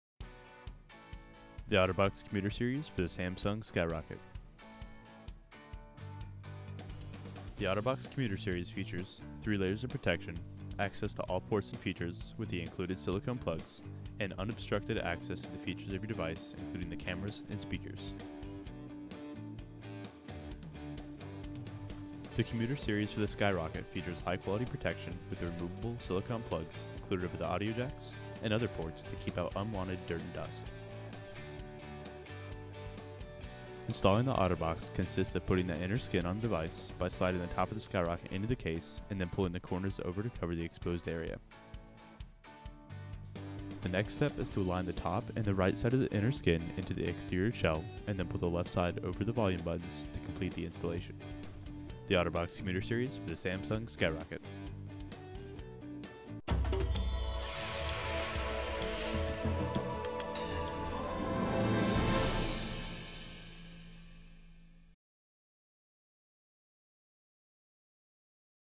OtterBox Commuter Series Case for Samsung Skyrocket
The OtterBox Commuter Series for the Samsung Skyrocket is a perfect on-the-go accessory for everyday handheld technology.

Safeguarding the screen from scratches and the corners from bumps and shocks, this style case offers a unique design with reliable strength.  Included is a self-adhering clear protective film to safeguard your Samsung Skyrocket touch screen.

Black
Part# A9594
Mnf# SAM4-I727XE3E4OTR

Black / Night Blue
Part# A9750
Mnf# SAM4-I727X20E4OTR